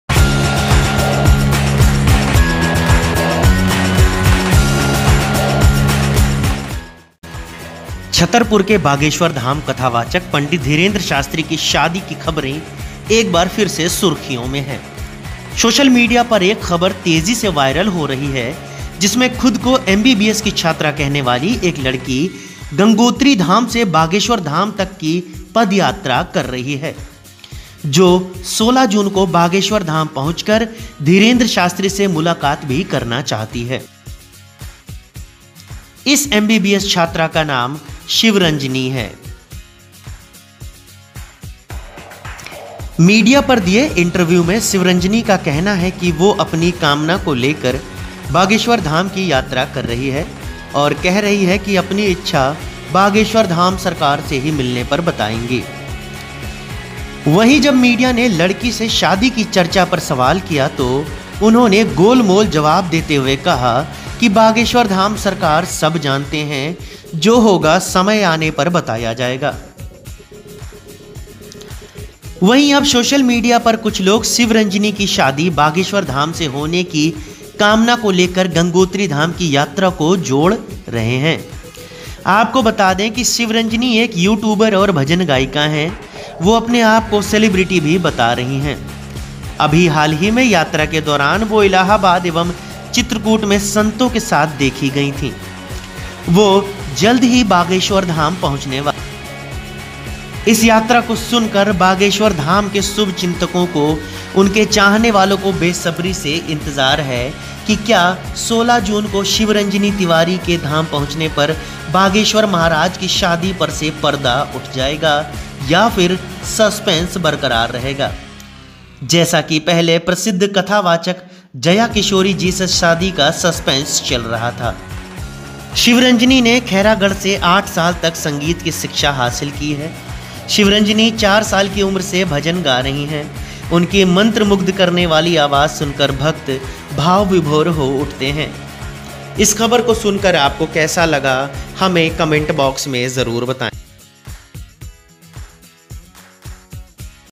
Bageshwar Dham Sarkar शादी करेगी MBBS स्टूडेंट ? | Dhirendra Shastri Marriage | News Update
Bageshwar Baba Marriage: ख़बरों के मुताबिक बागेश्वर बाबा से एक छात्रा शादी करना चाहती है एमबीबीएस की इस छात्रा ने गंगोत्री से बागेश्वर धाम तक सिर पर कलश रखकर पदयात्रा शुरू की है.

Shivranjani Tiwari Yatra : प्राणनाथ के लिए पदयात्रा, शिवरंजनी की कलश यात्रा | Bageshwar Dhamप्राणनाथ के लिए पदयात्रा. शिवरंजनी की कलश यात्रा. 16 जून को पहुंचेंगी शिवरंजनी तिवारी. बागेश्वर सरकार, दीवानगी अपार. पद यात्रा गंगोत्री धाम से बागेश्वरधाम तक. 16 जून को पहुंचेंगी शिवरंजनी तिवारी.